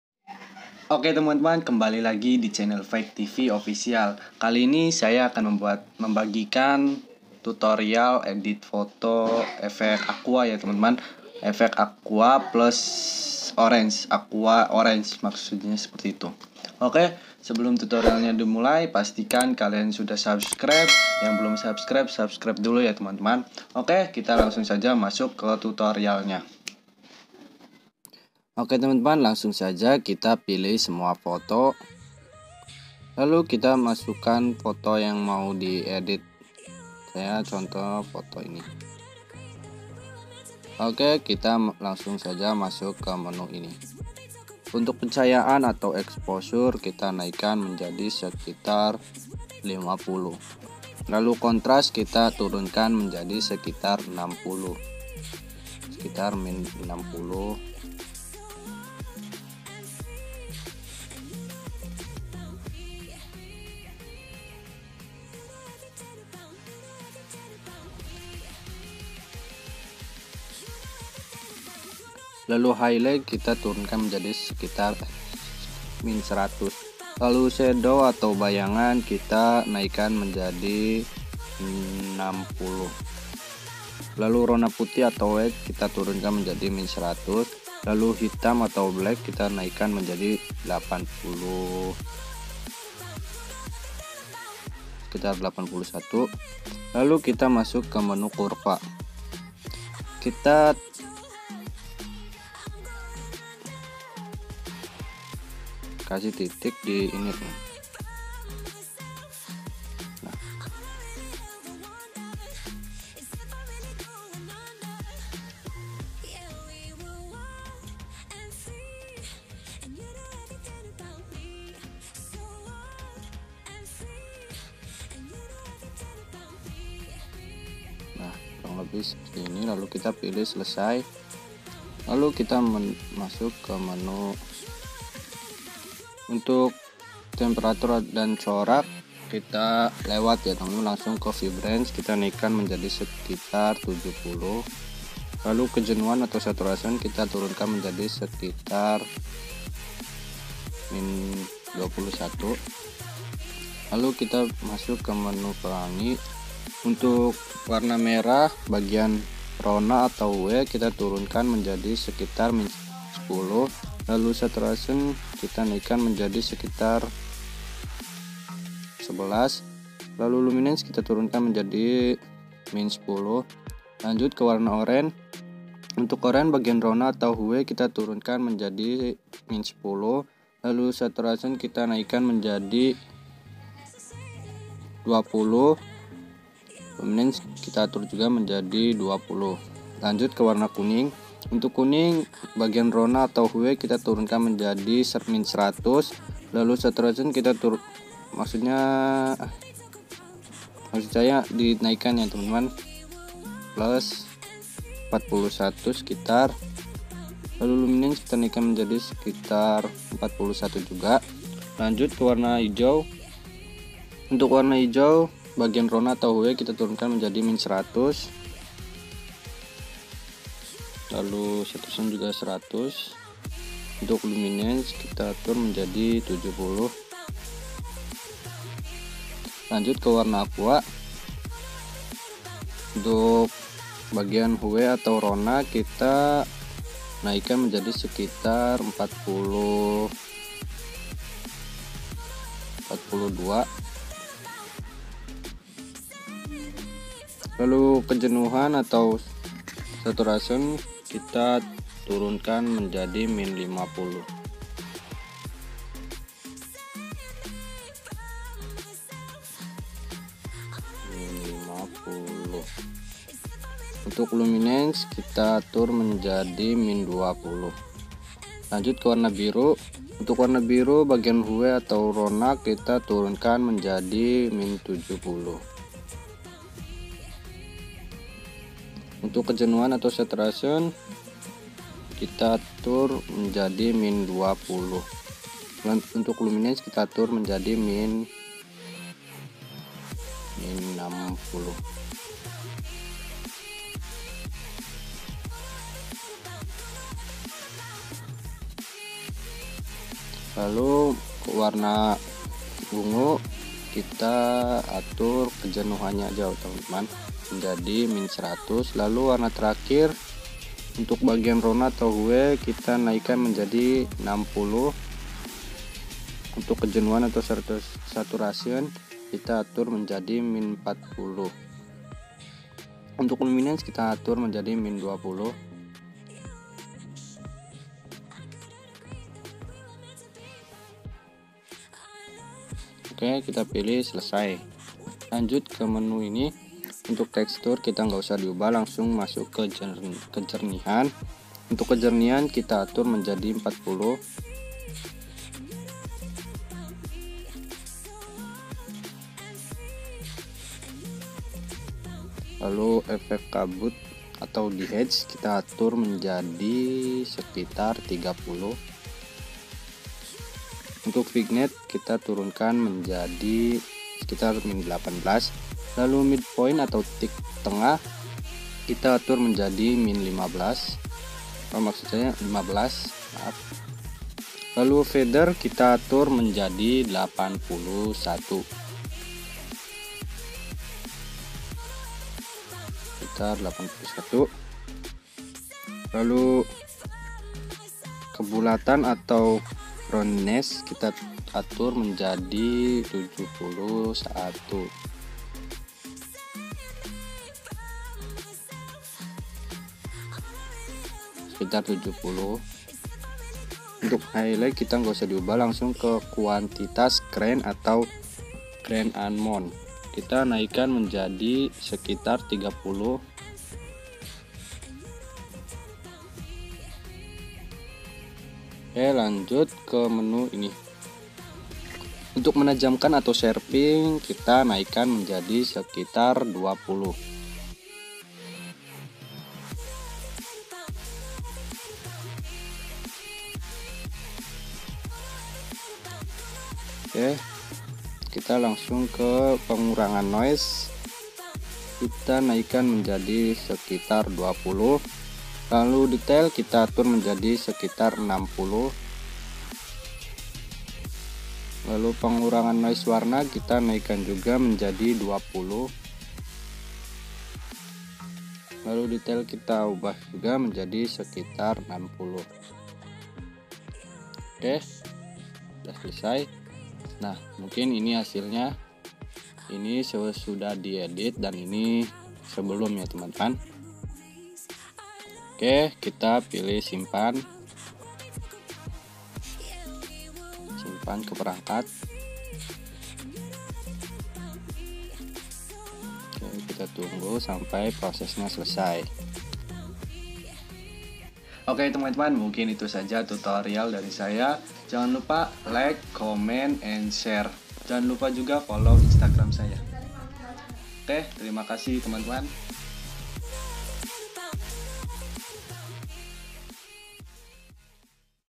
Tutorial Edit Foto Efect Aqua & Orange Lightroom
Tutorial Efect Aqua & Orange Lightroom
Oke teman-teman mungkin itu saja tutorial yang bisa saya berikan. Maaf jika ada yang kurang jelas dalam penyampaian saya